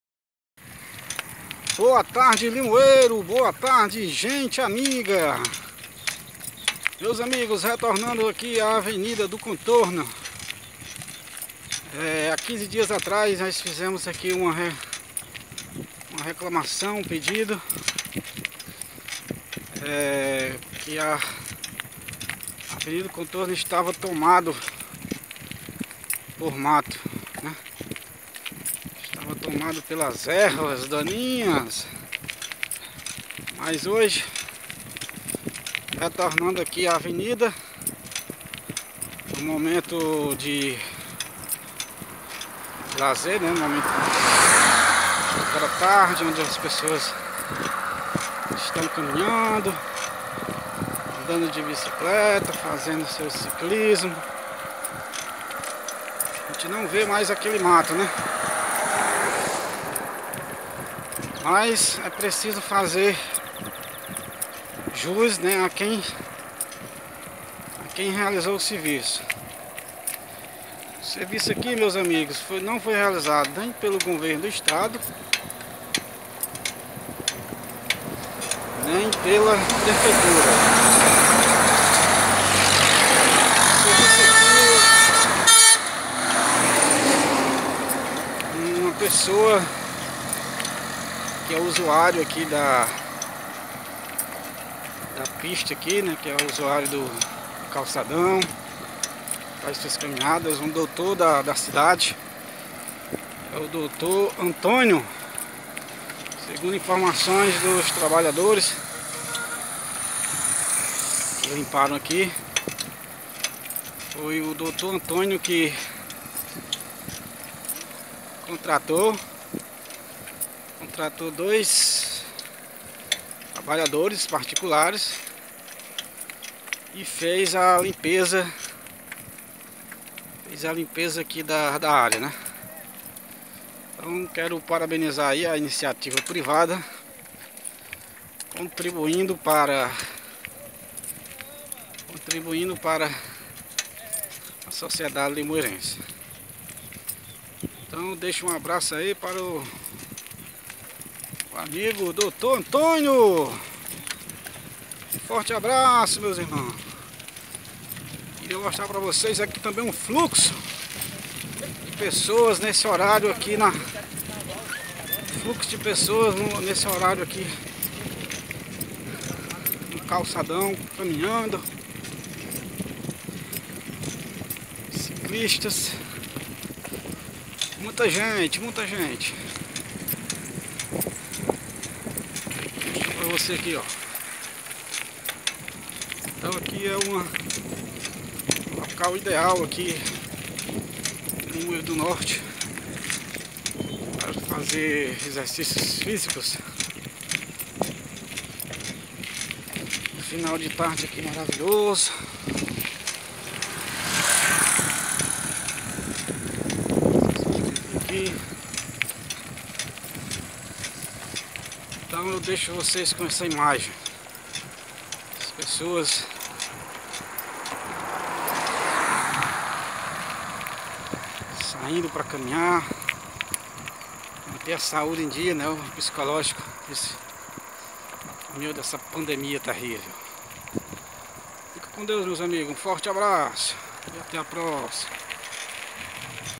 Iniciativa Privada Faz Operação de Limpeza na Avenida de Contorno em Limoeiro do Norte.
Limoeiro do Norte.  A avenida do contorno ao núcleo urbano de Limoeiro do Norte, possui 8,7 km de extensão. A avenida visa a melhorar o trânsito da região, sendo uma alternativa para aliviar o tráfego pesado de veículos do centro do município. Também estão incluídos no projeto pistas de rolamento, ciclovia, paradas de ônibus, sinalização, obras de drenagem e passeios adequados às normas de acessibilidade.

A avenida do Contorno tem início na rodovia CE-377 (que é ligada a Tabuleiro do Norte), segue na direção leste do município e conecta-se novamente à rodovia na saída de Limoeiro do Norte para o distrito de Flores.

Fonte: Governo do Estado do Ceará